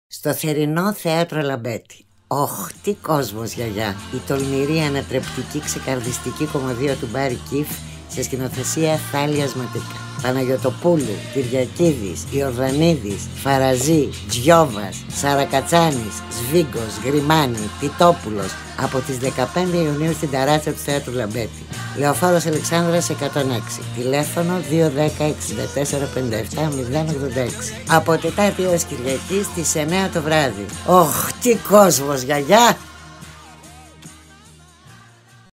"Ωχ! Τι κόσμος γιαγιά! / Radio Spot
Από τη μια η οικογένεια της φοβερής γιαγιάς Σπράϊτλυ (μια φτωχή οικογένεια, που ζει στήνοντας κομπίνες με ...κοκορομαχίες), η εγγονή της, που βρίσκεται μόνιμα υπό την επήρεια ηρεμιστικών χαπιών, ο εγγονός της ο οποίος είναι ...πυγμάχος και εξαιρετικός στο να "στήνει αγώνες"). Από την άλλη ο κόσμος του μεγαλοαστού Οράτιου Κλώτον (σεξομανής, διεφθαρμένος πολιτικός). Στη μέση ο σκανδαλοθήρας δημοσιογράφος, ο πληρωμένος αστυνομικός που θέλει απεγνωσμένα ένα δάνειο, ο εργατοπατέρας, η αφελής νεαρά πλουσία που κάνει αγαθοεργίες, ο ξεπεσμένος και αλκοολικός γιατρός, ο φουκαράς, τοξικομανής καλλιτέχνης. Ένα στημένο ατύχημα, που καταλήγει να γίνει δυστύχημα, ενώνει τους δρόμους τους και... τους μπερδεύει... αρκετά! Όλα αυτά, κάπου μέσα στη δεκαετία του 80...

Τολμηρός και βωμολόχος ο βραβευμένος και πολυγραφότατος Βρετανός σατιρικός, Μπάρυ Κηφ, "ακρωτηριάζει" όλες τις κοινωνικές τάξεις και "φωτογραφίζει" την ανθρώπινη Λούμπεν διάθεση. Πρόκειται για διασκευή του ομότιτλου έργου του Τόμας Μίντλετον που γράφτηκε το 1605.
Ο Κηφ θέτει την εξέλιξη του έργου και μεταφέρει πρόσωπα και καταστάσεις στη δεκαετία του 80. Την πρώτη απόπειρα της διασκευής του ο Κηφ την έκανε το 1977 αλλά απαγορεύτηκε καθώς χαρακτηρίστηκε "ακραία" και "επικίνδυνη". Το 1984 ο συγγραφέας επανήλθε και σήμερα το έργο θεωρείται μία από τις καλύτερες και πλέον κλασσικές κωμωδίες του Ηνωμένου Βασιλείου.

ΣΥΝΤΕΛΕΣΤΕΣ
Μετάφραση : Ερρίκος Μπελιές
Σκηνοθεσία : Θάλεια Ματίκα
Μουσική - μουσική επιμέλεια : Παναγιώτης Τσεβάς
Σκηνικά : Γκάι Στεφάνου
Κοστούμια : Μάρλι Αλειφέρη
Φωτιστικός σχεδιασμός : Μπάμπης Αρωνης

ΠΡΩΤΑΓΩΝΙΣΤΟΥΝ
Άννα Παναγιωτοπούλου, Βλαδίμηρος Κυριακίδης, Τάσος Ιορδανίδης, Σοφία Φαραζή, Ορέστης Τζιόβας, Γιάννης Σαρακατσάνης, Σταύρος Σβήγκος, Τερέζα Γριμάνη, Μιχάλης Τιτόπουλος.

ΘΕΡΙΝΟ ΘΕΑΤΡΟ ΛΑΜΠΕΤΗ
Ημέρες και ώρες παραστάσεων: Τετάρτη-Κυριακή στις 21.00.
Τιμές Εισιτηρίων: €18 και φοιτητικό €12